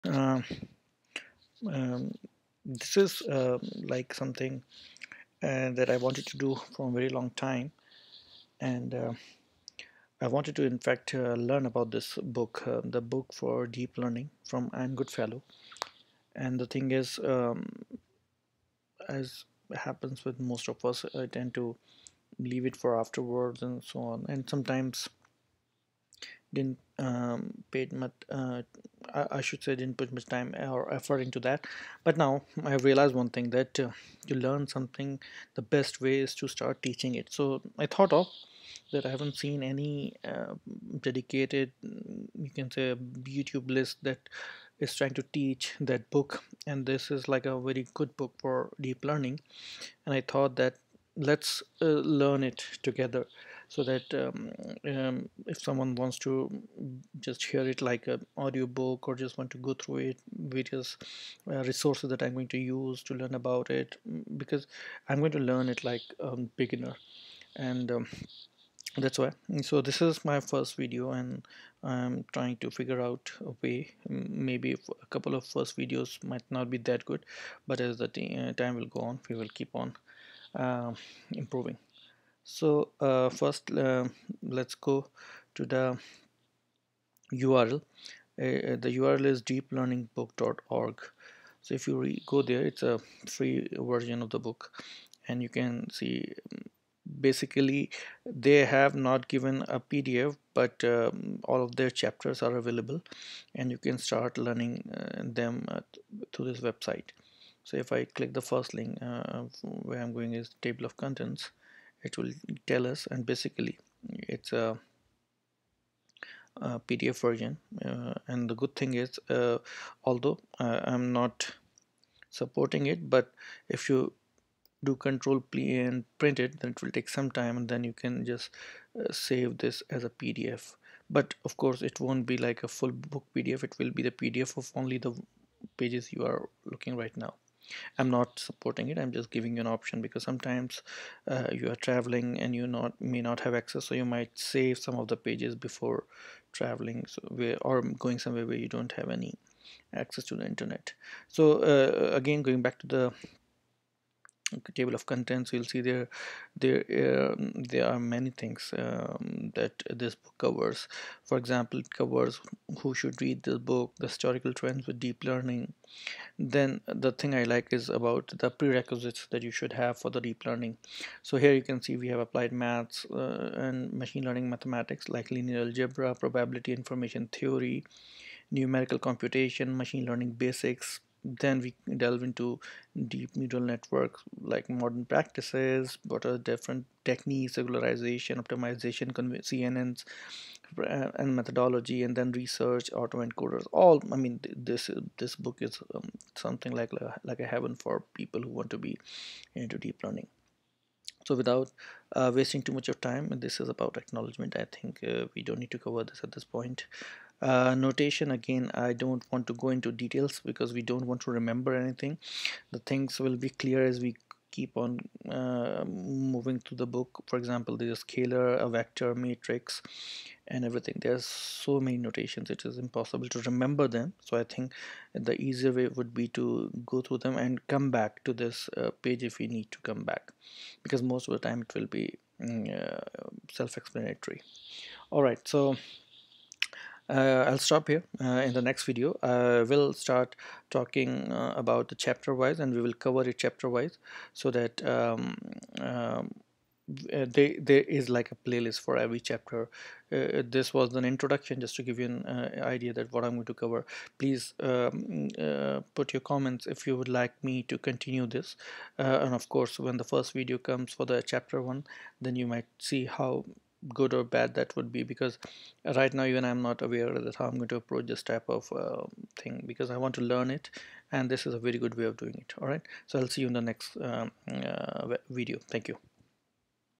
Learning Deep Learning from Ian Goodfellow
In this first introductory video, we will provide a brief overview of the book and its contents, as well as the format of our channel and the resources we will be using to supplement our understanding of the material. We will also discuss the prerequisites for understanding the concepts covered in the book and suggest some additional resources for those who are new to the field of machine learning. Whether you're a student, a researcher, or just someone with a general interest in machine learning, this channel is for you! Join us as we delve into the world of deep learning and explore the latest advancements in the field.